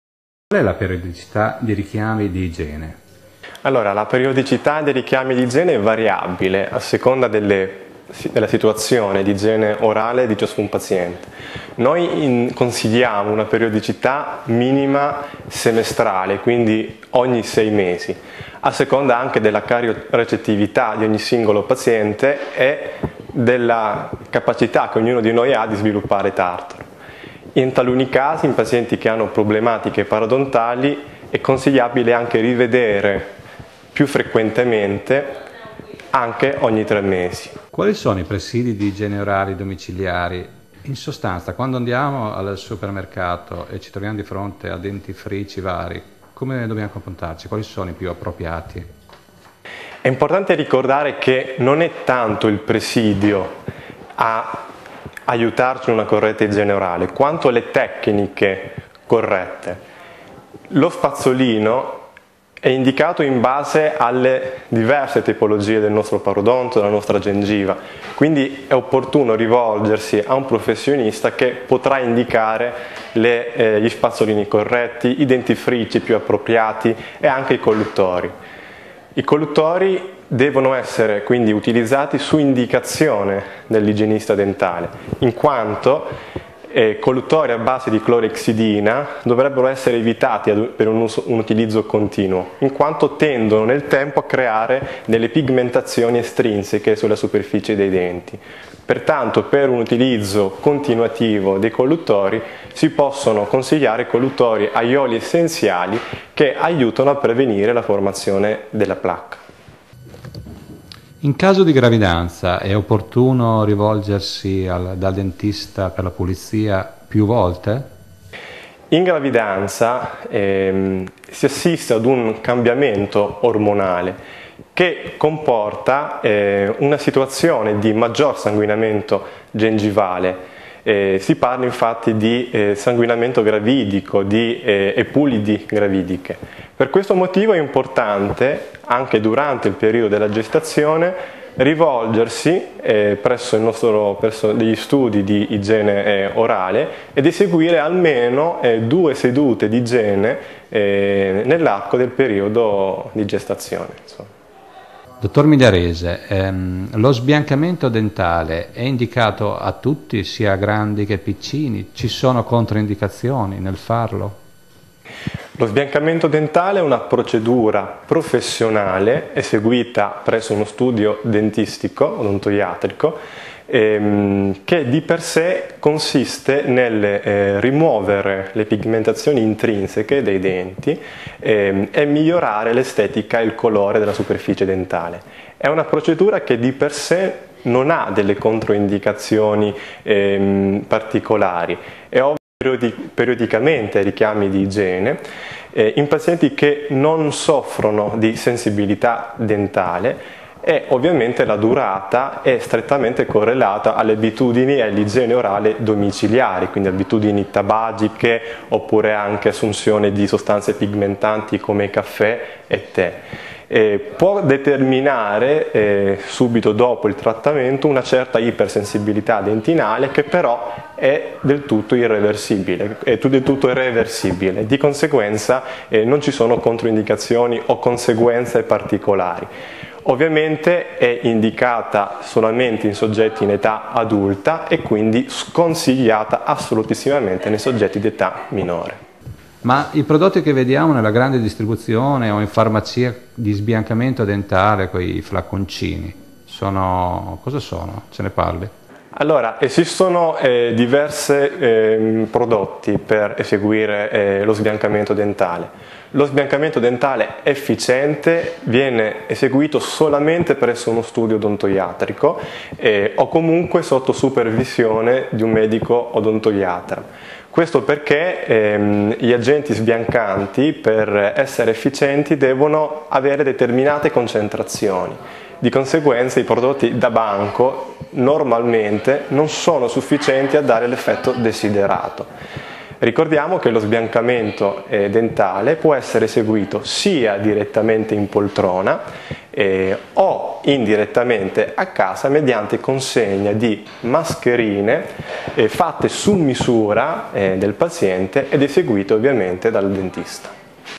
Come si puliscono i denti ? Ecco come fare per denti in ordine
Ogni quanto bisogna pulire i denti, quali sono i migliori dentifrici, spazzolini e collutori. Quando devono recarsi da dentista le donne in gravidanza?